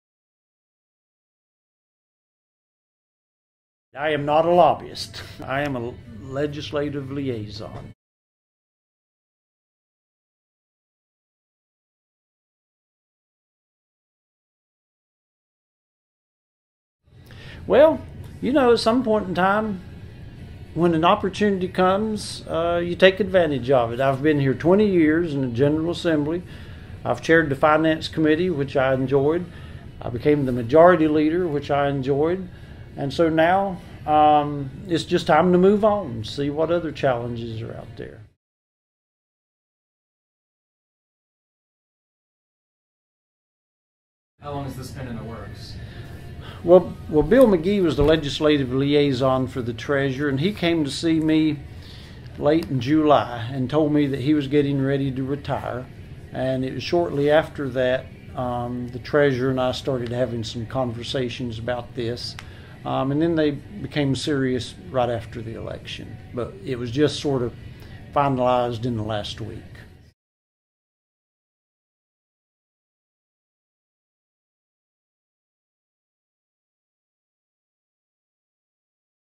Rep. Edgar Starnes resigns
Longtime N.C. House member Edgar Starnes, a Republican from Caldwell County, is resigning effective Jan. 13 -- one day before the start of the 2015 General Assembly -- to become the legislative liaison for the state treasurer's office. This video interview was conducted Jan. 12 in his Raleigh office.